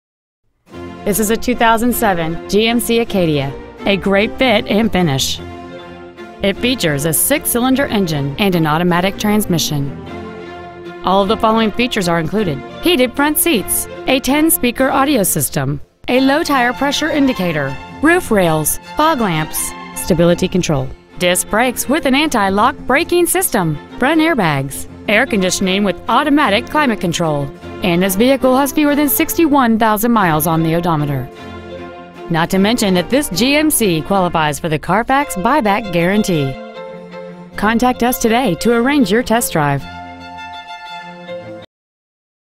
Used 2007 GMC Acadia Miami FL
Year : 2007
 Make : GMC
 Model : Acadia
 Engine : dohc vvt v6
 Trans . : automatic
 Exterior : gray
 Miles : 60,580
 Interior : gray

 Lehman Mazda